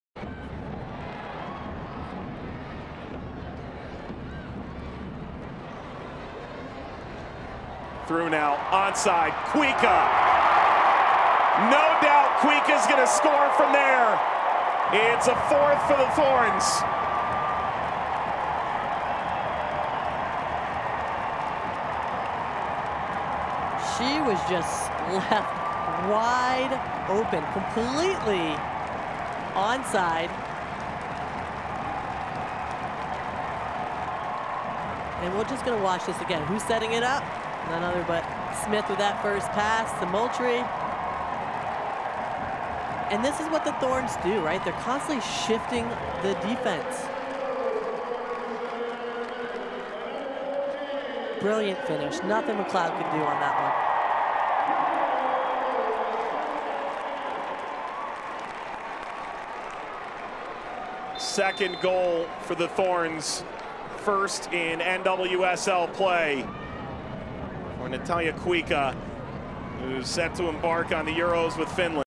GOAL | Natalia Kuikka with a nifty finish against the Pride
Thorns FC's Natalia Kuikka with a nifty finish against Orlando in Portland's 6-0 win.